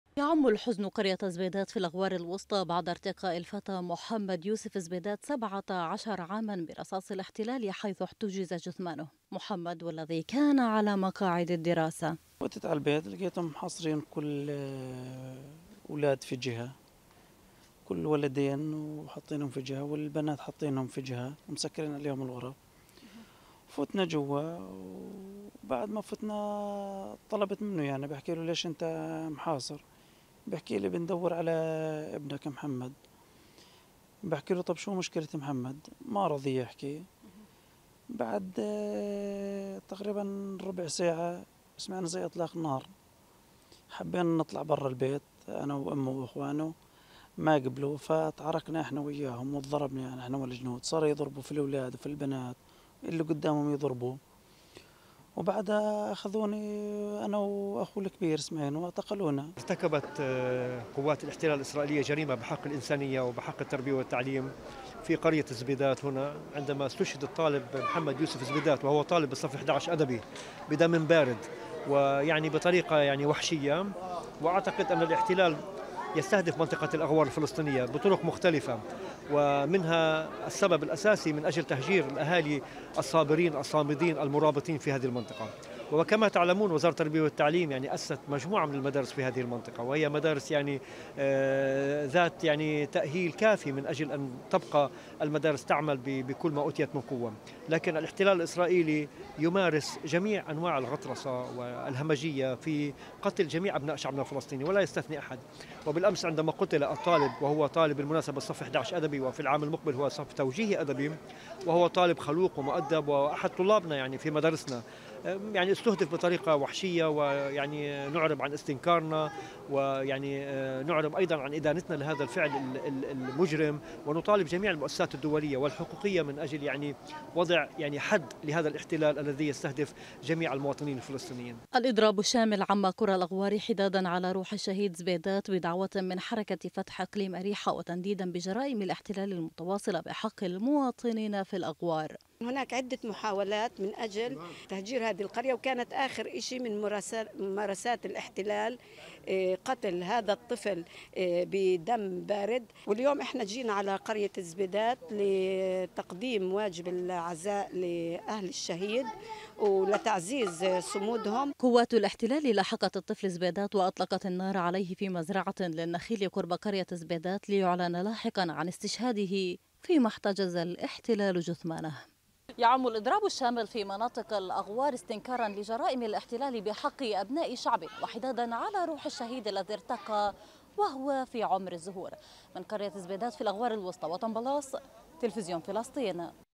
الإضراب الشامل يعم قرى الأغوار حدادا على روح الشهيد الطفل محمد زبيدات
لمتابعتنا على وسائل التواصل الاجتماعي